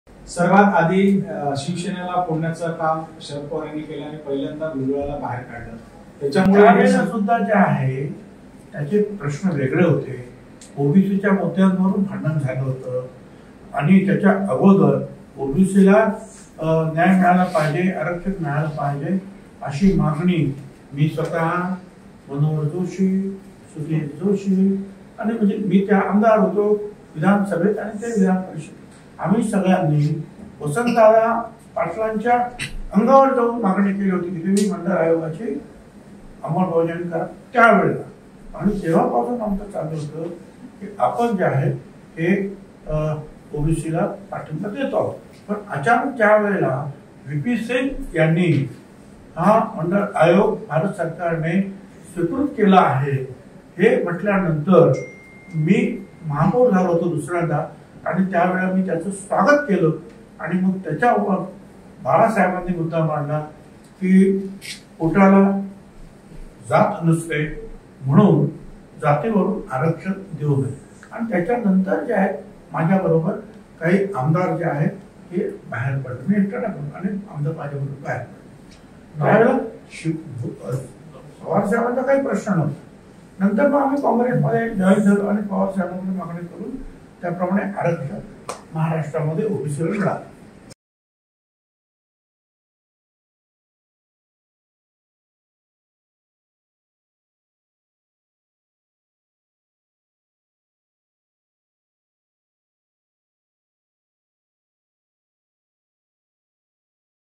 Chhagan Bhujbal | ओबीसी आरक्षणावर काय म्हणाले भुजबळ | OBC Reservation
ओबीसीला आरक्षण मिळाले, आमचा नेहमीच या मागणीला पाठिंबा दिला असल्याची प्रतिक्रिया राष्ट्रवादीचे नेते छगन भुजबळ यांनी दिली आहे.